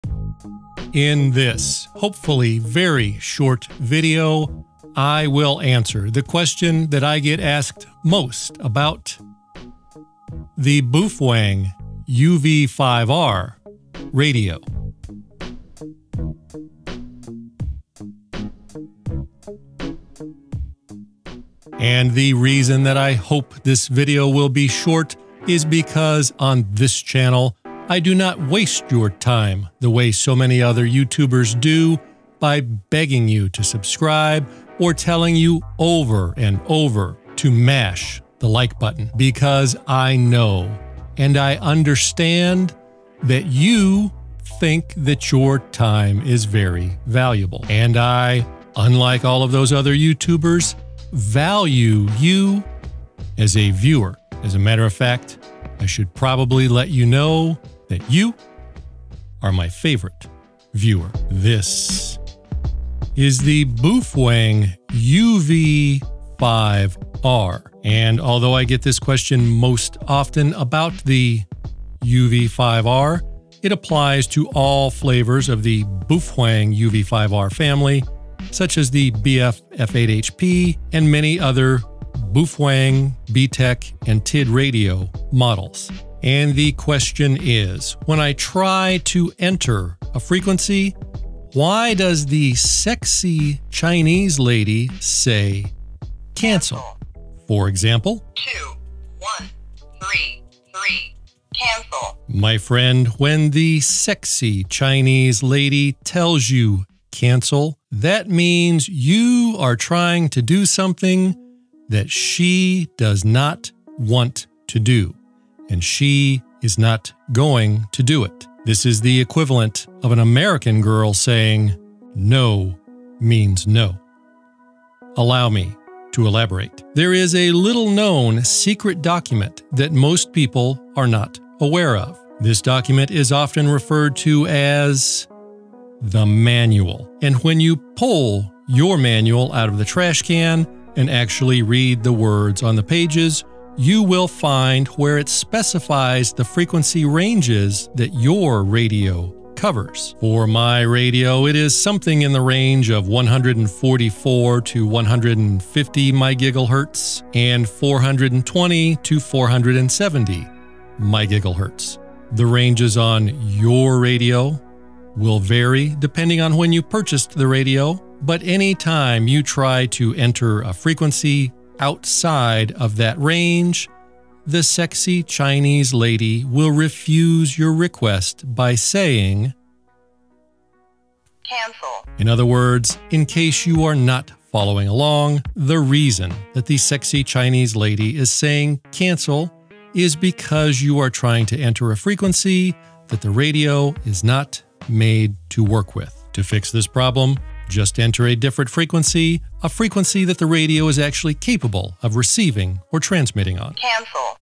Baofeng UV-5R: Why Does The UV-5R Keep Saying "Cancel"? What Cancel Means On A Baofeng Ham Radio
In this very short video I answer the most common question I get about the Baofeng UV-5R ham radio, and similar radios - "Why does it keep saying CANCEL"?   I explain what 'cancel' means, and why the Baofeng says "cancel". Please never forget, the UV-5R, like many Baofeng radios are ham radios - Always follow all FCC guidelines when using your ham radio!